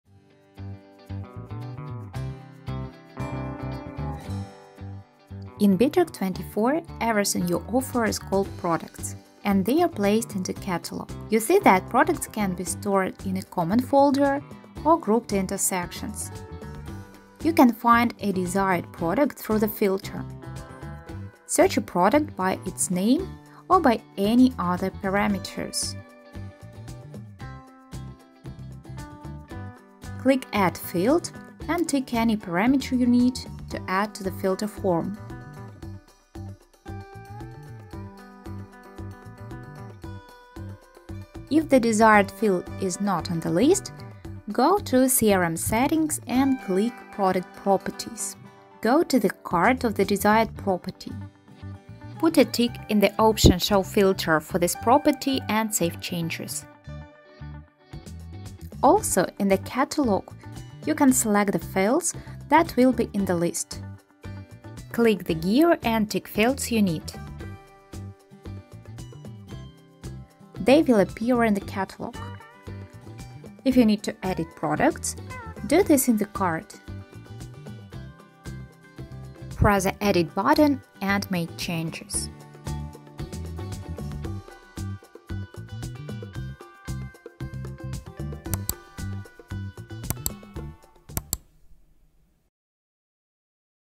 Commercial catalog organization
In Bitrix24 there is no division into products or services. Everything you offer is called Products. They are stored in the catalog.